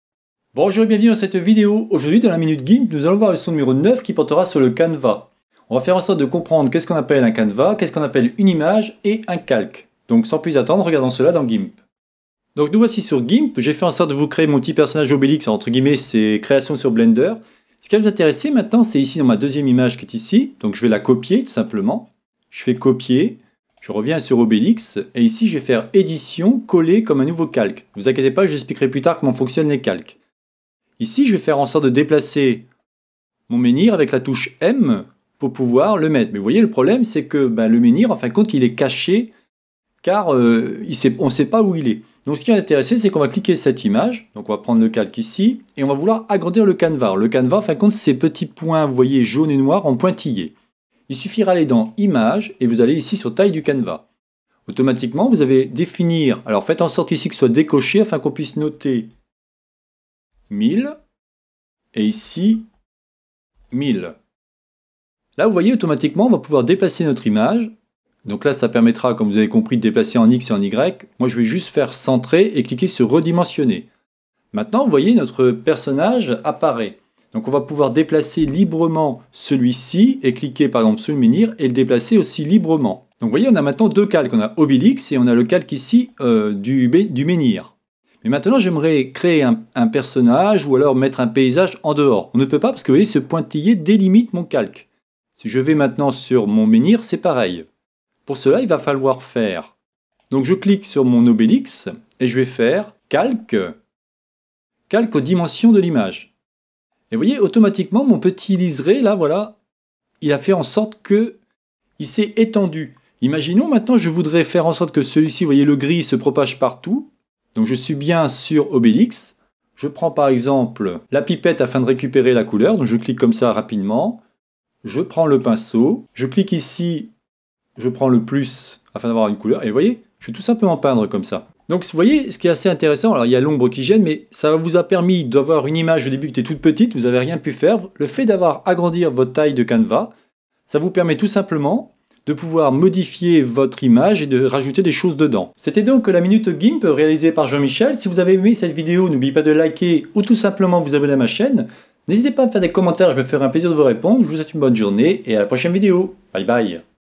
Gimp - 09 - Canevas
Dans cette vidéo, nous allons apprendre à travailler sur le canevas avec 2 calques où l'on trouvera Obélix et son menhir.

Plus d’infos:

Lien google plus - Hashtags Gimp